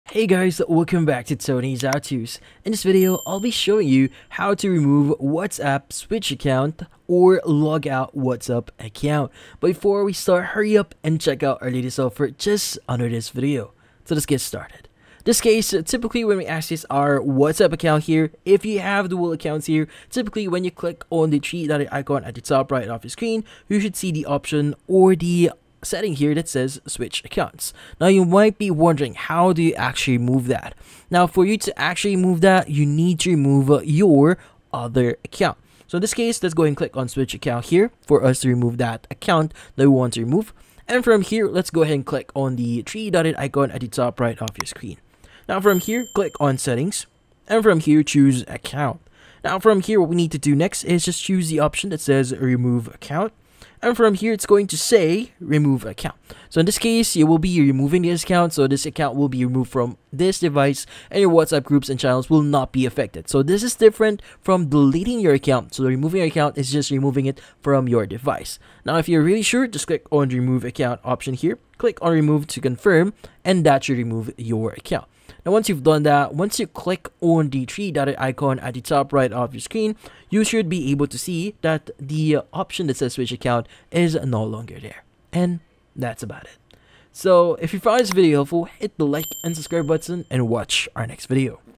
How to Remove WhatsApp Switch Account | Logout WhatsApp Account (2024)
In this video, I show you How to Remove WhatsApp Switch Account | Logout WhatsApp Account in 2024. The quick and easy tutorial how to switch whatsapp account on same phone. Do you want to know step by step how to delete whatsapp switch account in 2024? Yes? Great!